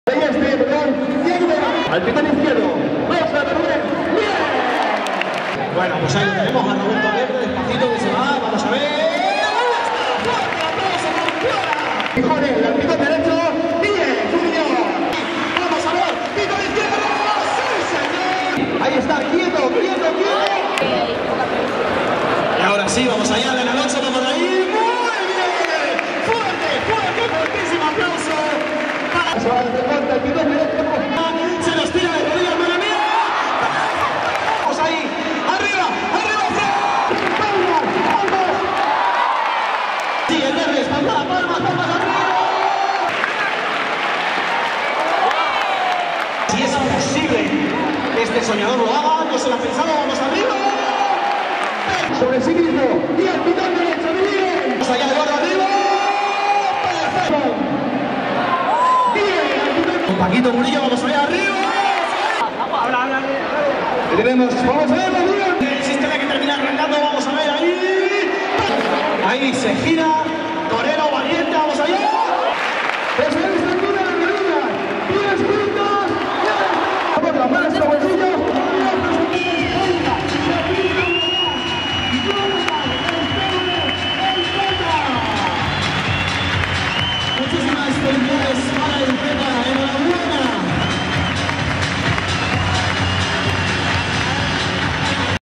La final del concurso de recortadores de San Fermín
Jonatan Estébanez se ha proclamado vencedor de esta edición